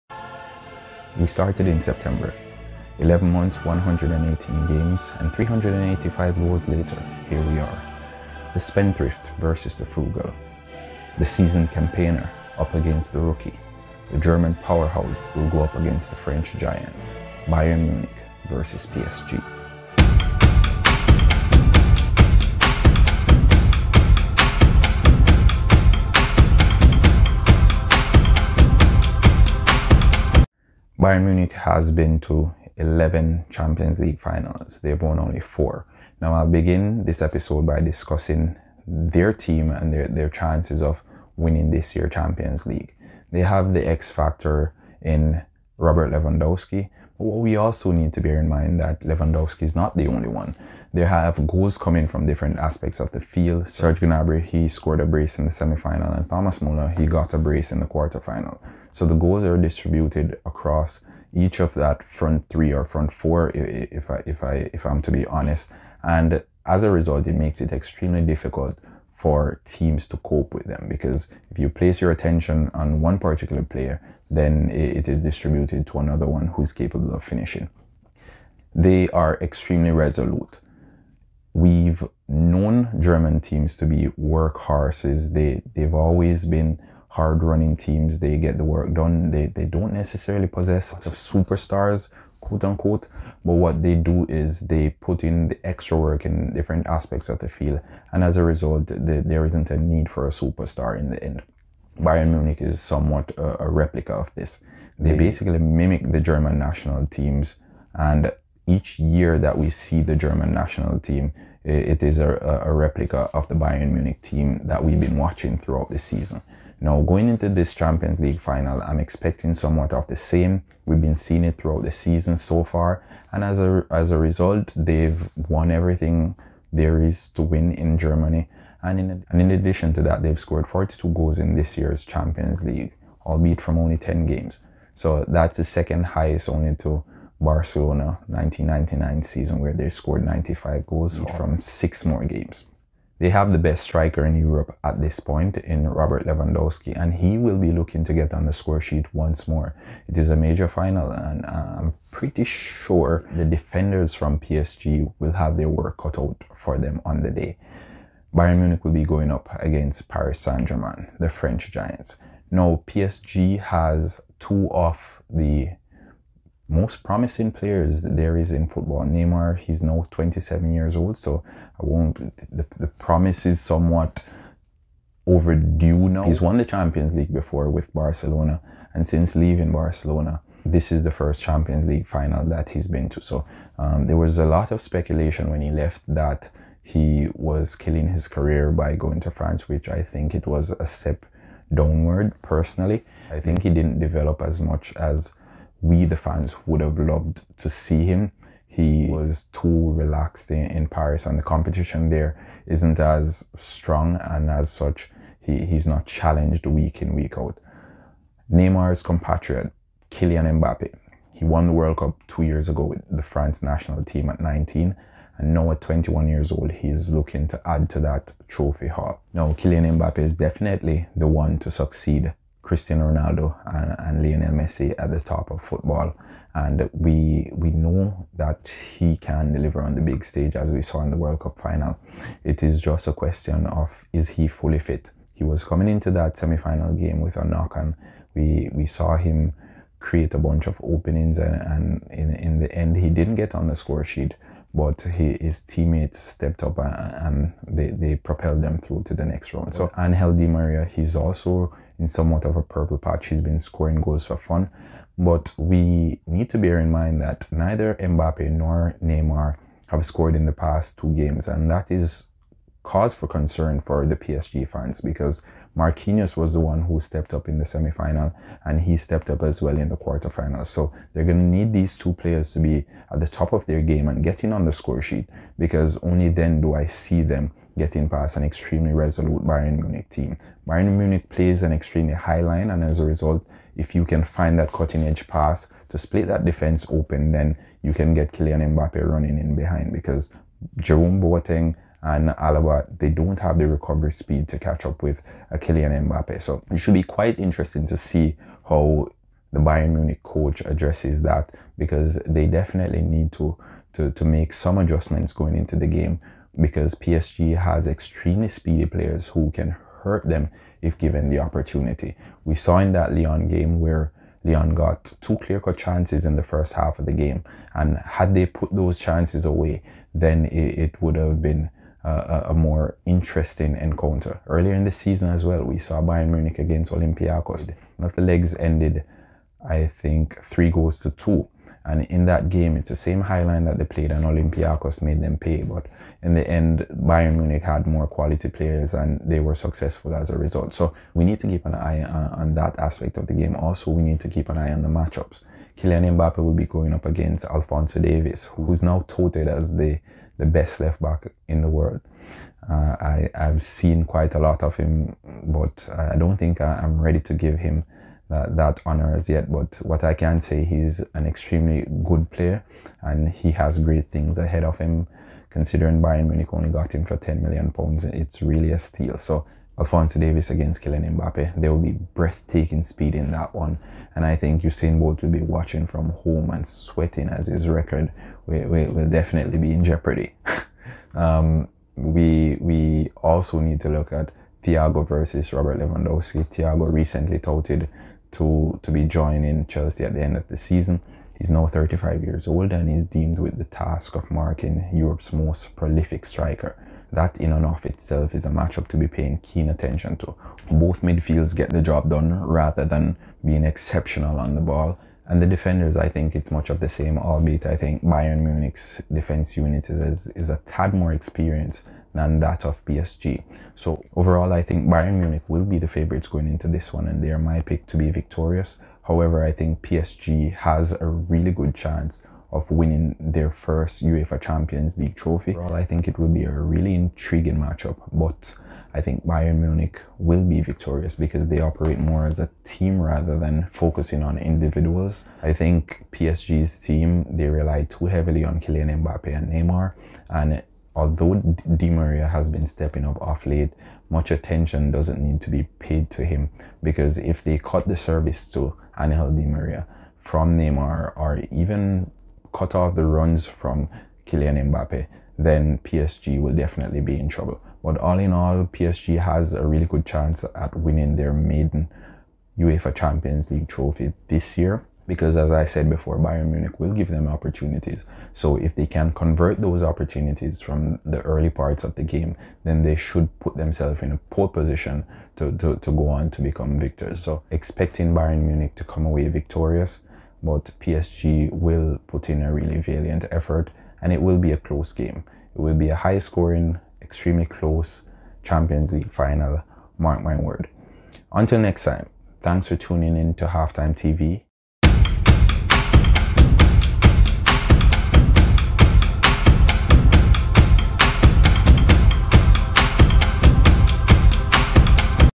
PSG vs BAYERN MUNICH| CHAMPIONS LEAGUE FINAL| HALFTIMETV Ep.19
Host Norando Brown outlines key areas where game winner might come from as well as matchups to look out for going into Sunday's Champions League Final. NeymarJr. has 90mins to quieten all the naysayers who questioned his move a few years ago. Norando posits that Thiago Silva against Robert Lewandowski is one to look out for as well as Kylian Mbappé against Alphonso Davies. The final football game of the 2019/2020 campaign promises fireworks

Follow Norando on IG @d_lastgodfather
And our Page @halftime_tv